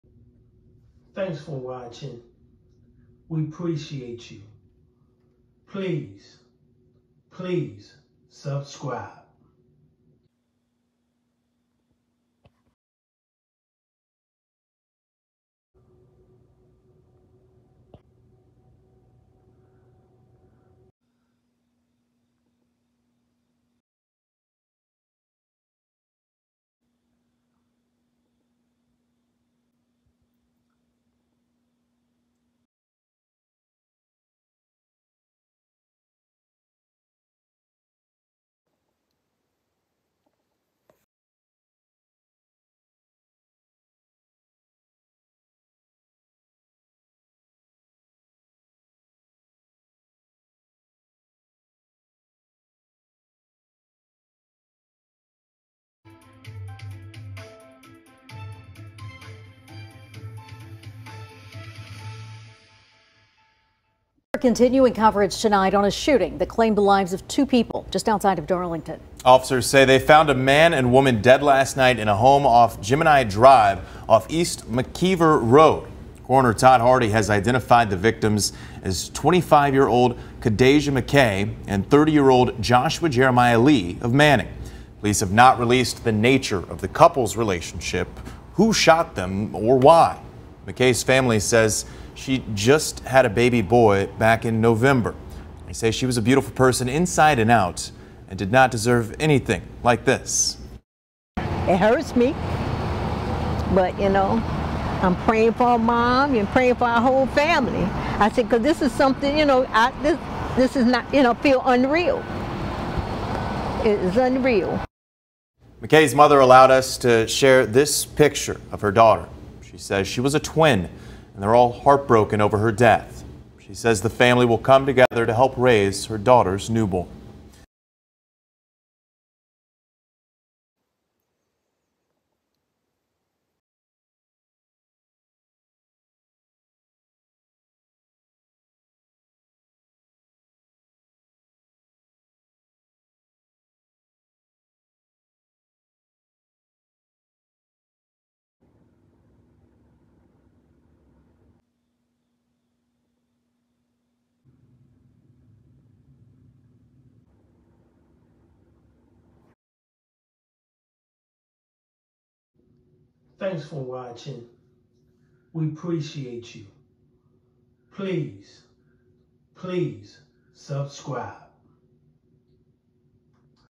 DARLINGTON, SOUTH CAROLINA JAN 2, 2023, KADAJAH McKAY 25 SHOT KILLED IN MURDER-SUICIDE AT HOME!
GO TO:   YOUTUBE.COM/@REALWOMENMURDERSNOTFICTION9264